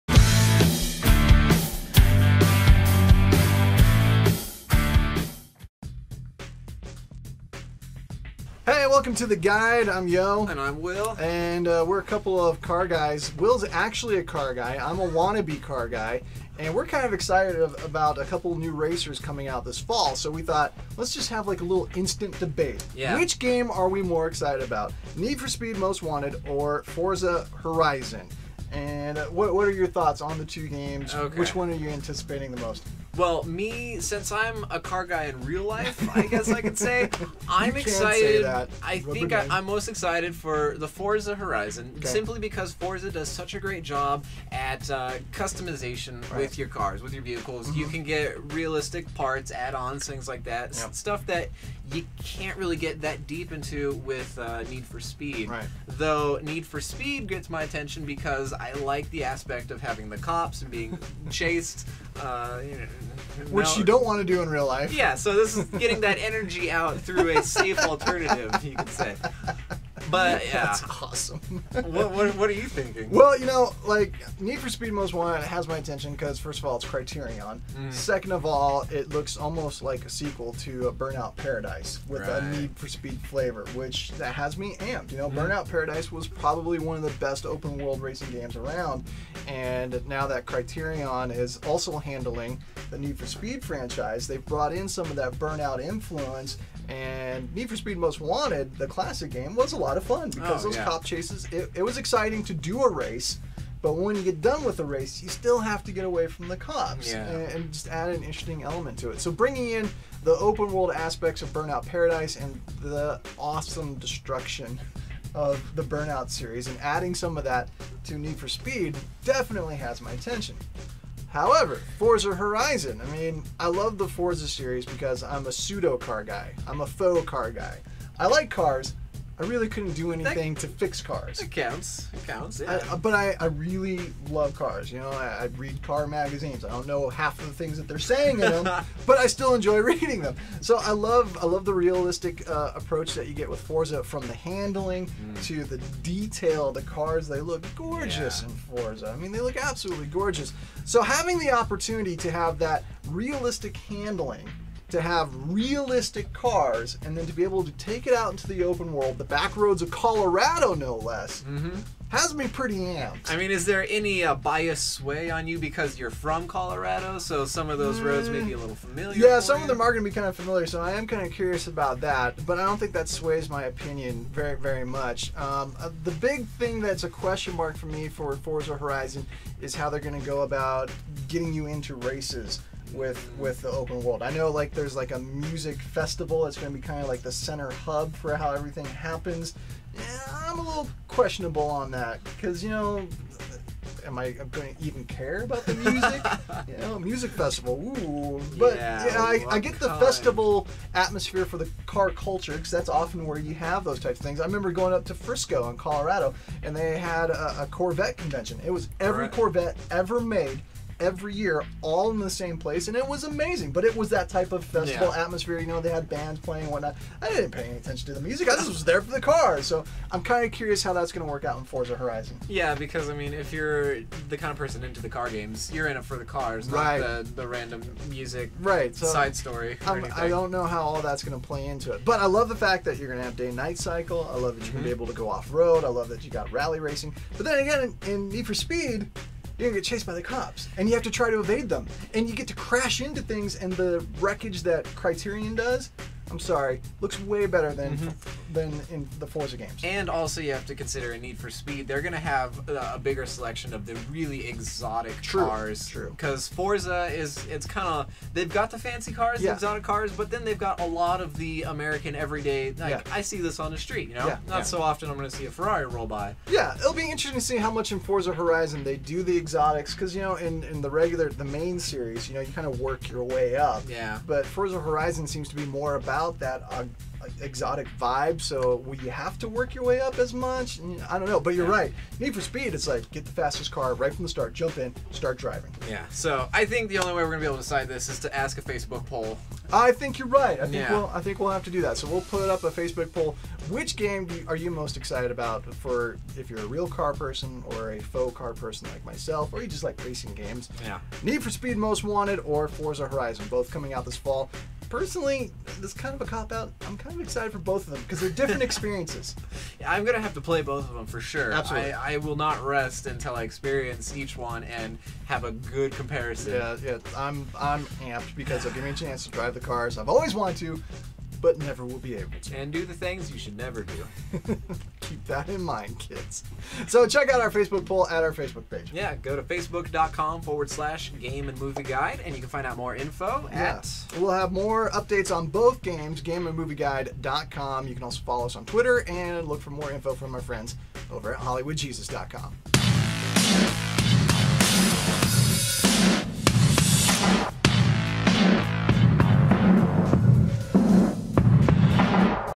7/26/2012 - Instant Debate: Forza Horizon vs. NFS: Most Wanted
Here at The Guide, we love cars. Will can actually do something with them while Yo just thinks they're cool. So this is a great year for all car lovers, real and faux, as Need For Speed: Most Wanted and Forza Horizon both release this Fall. The question is, which game is right for the car lover in you? Time for an instant debate.